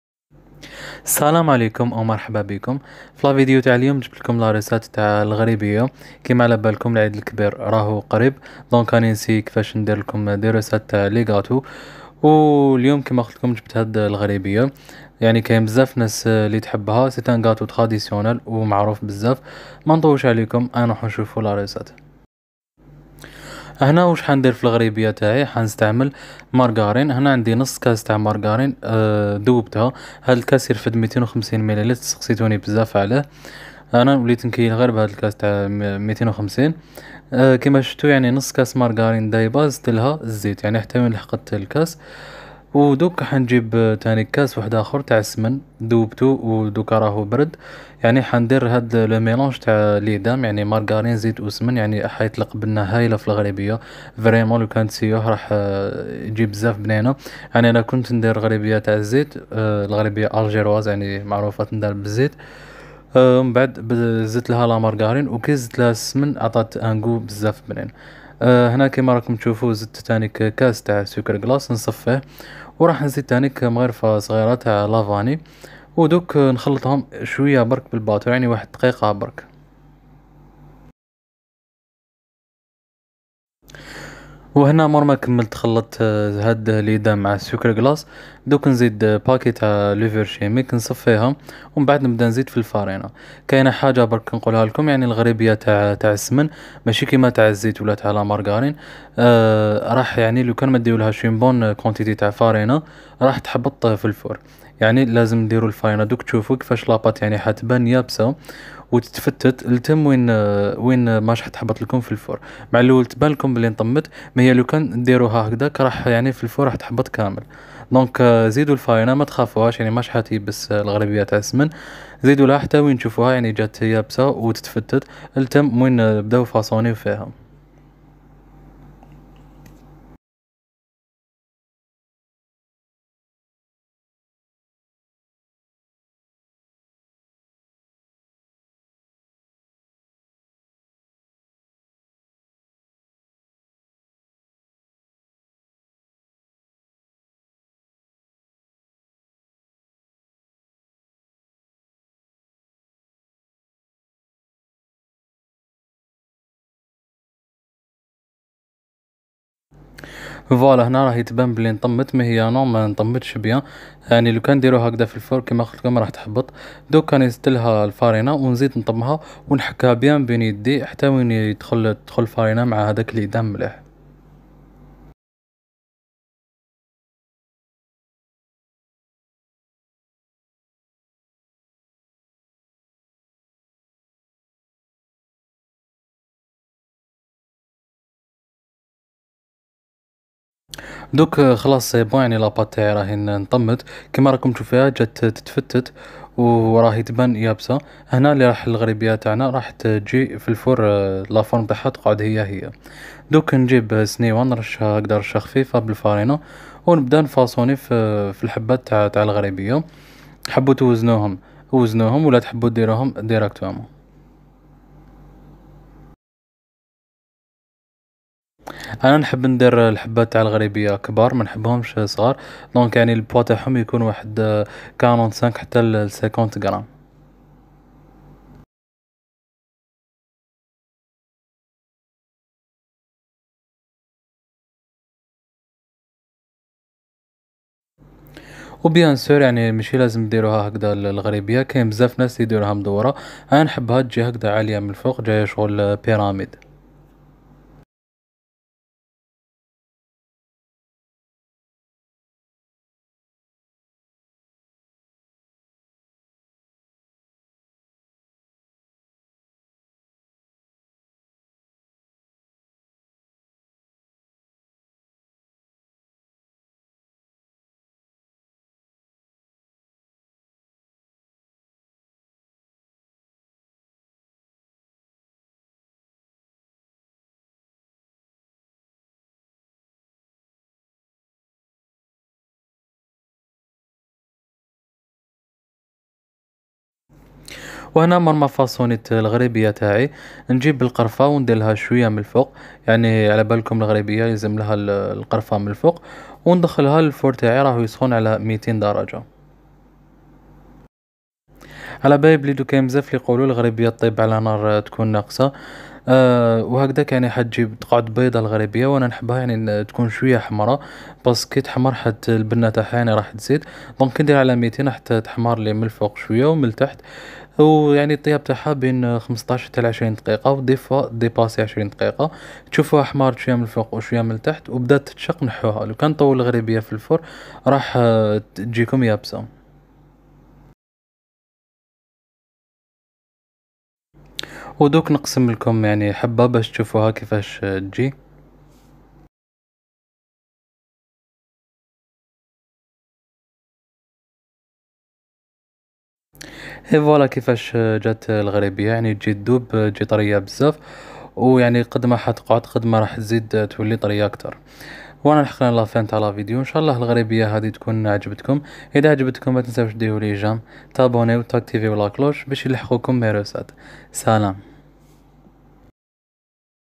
غريبية العيد حلوة جزائرية تقليدية سهلة و رائعة Ghribiya un gâteau traditionnel algérien
Ingrédients :
1 verre moitié margarine fondue et moitié huile
1 verre de smen fondue et refroidi
1 verre de Sucre glace
1 cac de vanille
1 paquet de levure chimique
farine
cannelle

المقادير:
كأس نصفه مارغرين ذائبة و نصفه زيت
كأس من ذائب و بارد
كأس سكر بودرة
ملعقة صغيرة فانيليا
كيس خميرة كيميائية
فرينة
قرفة